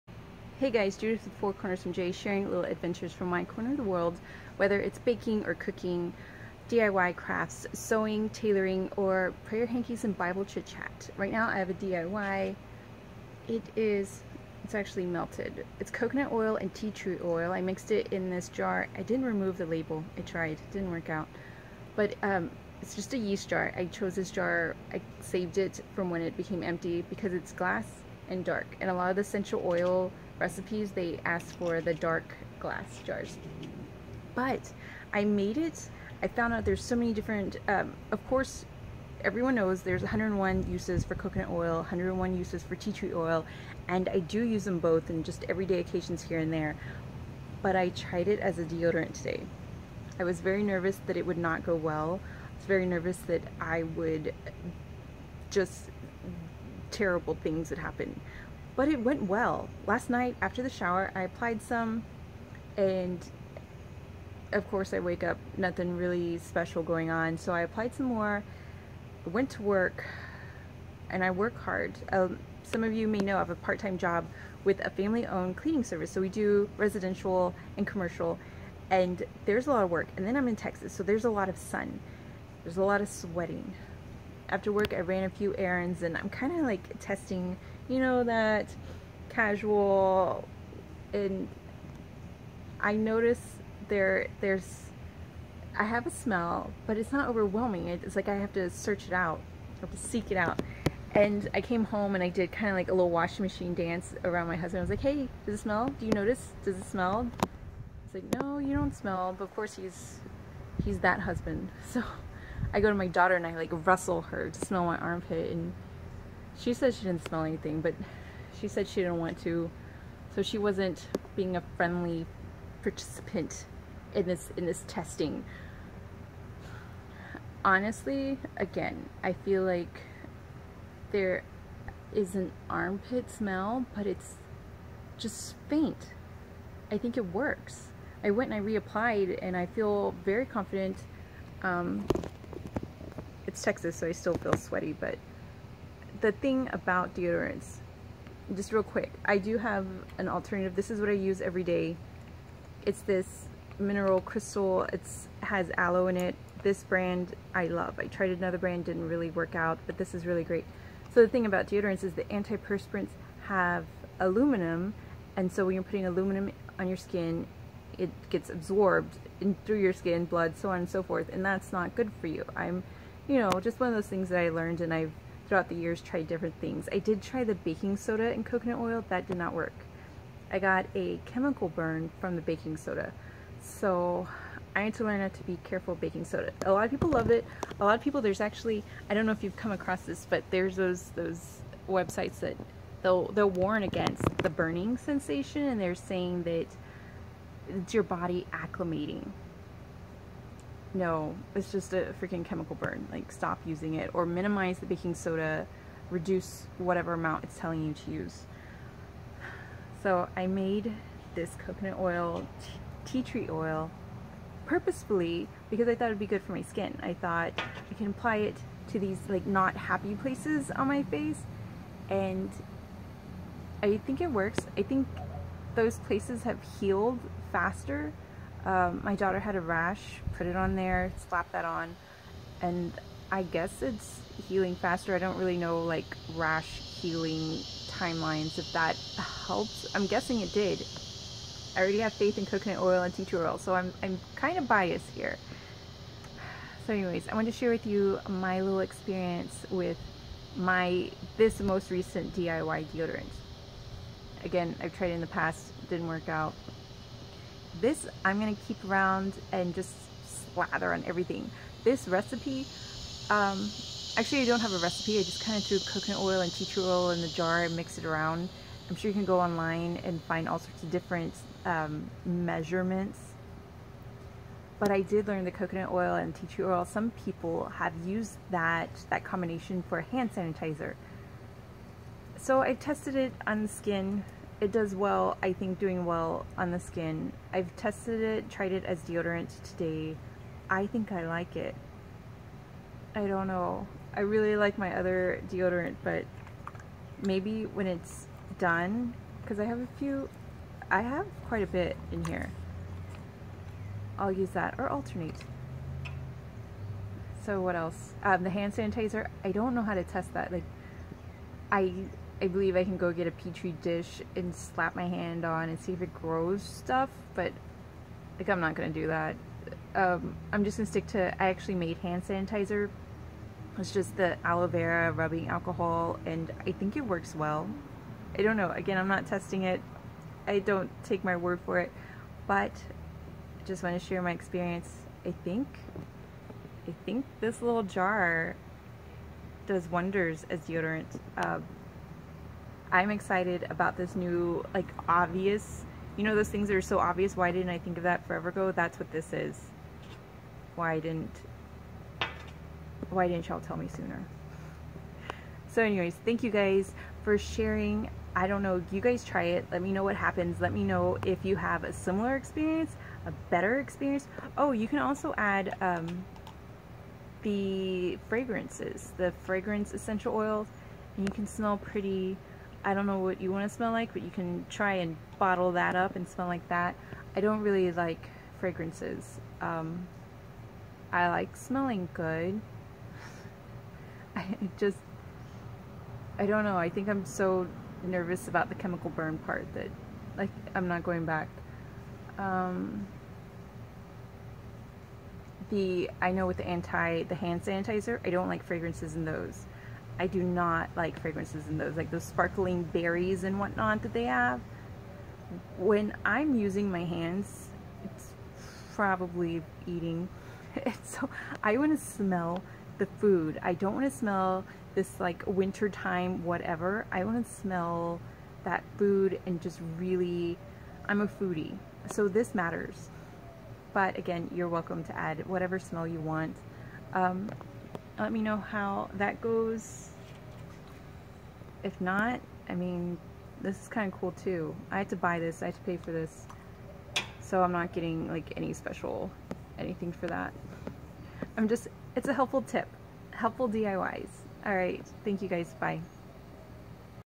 Reaction to my new DIY Deodorant
I tried a new DIY skin creme, and I began to wonder, "Would this also function as a deodorant?!"
I think it can!  It's only day one, but I think I'm sold.  Coconut oil + tea tree oil for everything!  Oral care, skin care, and now armpit care 💕 I don't know why I haven't tried this before 😮